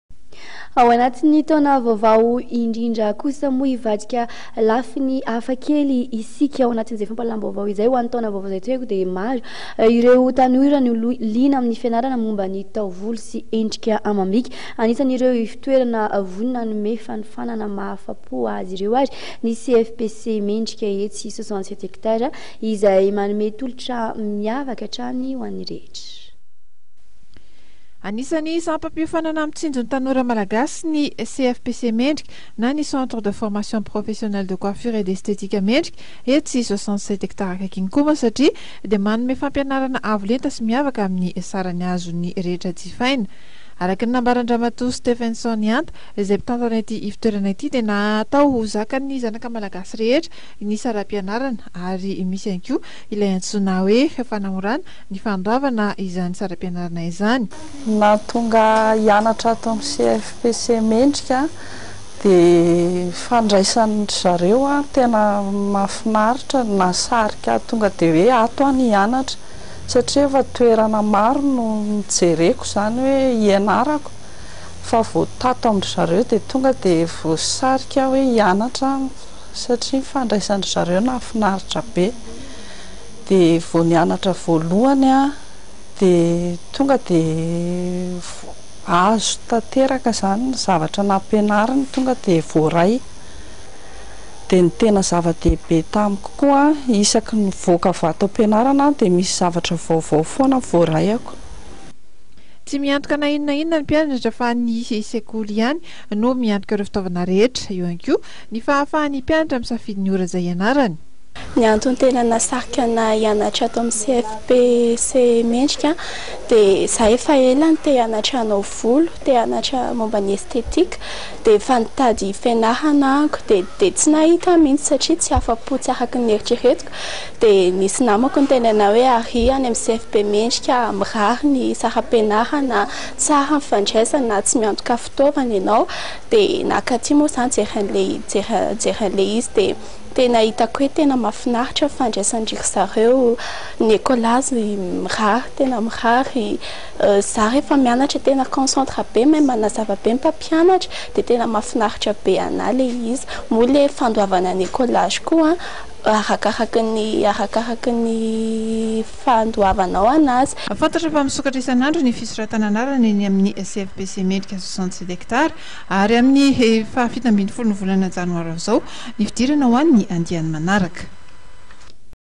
13 ASA
Rehefa manomboka ny taona dia maro ihany koa  ireo tanora no liana amin'ny fianarana momba taovolo sy endrika amam-bika. An'isan'ireo ivontorana vonona hanome fanofanana mahafapo azy ireo ny CFPC Mendrika etsy 67 izay manome tolotra miavaka amin'ny rehetra.